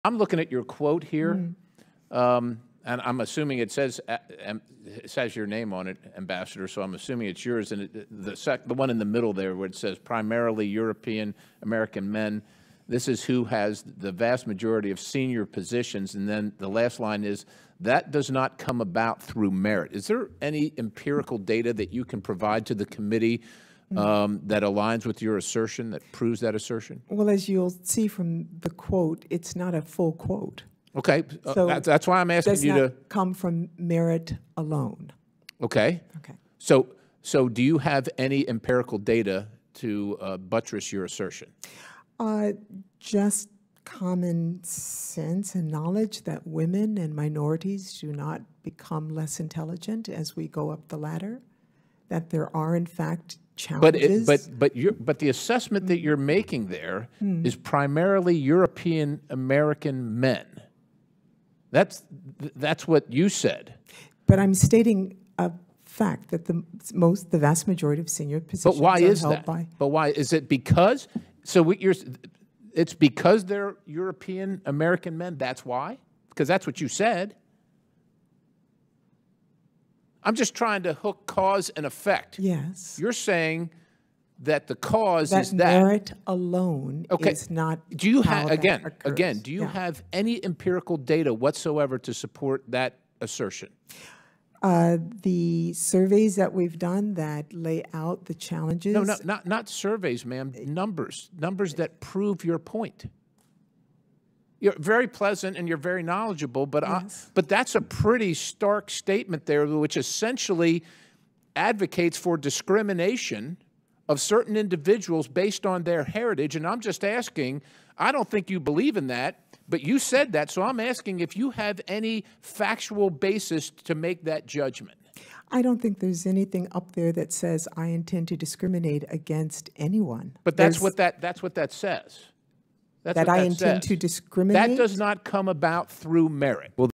Republican accuses Senior State Official of Discriminating Against White Men
U.S. Rep. Scott Perry (R-PA) misquotes Gina Abercrombie-Winstanley, a senior State Dept. official, and accuses her of discriminating against white men.

She's forced to correct him multiple times: "There's nothing there that says I intend to discriminate against anyone."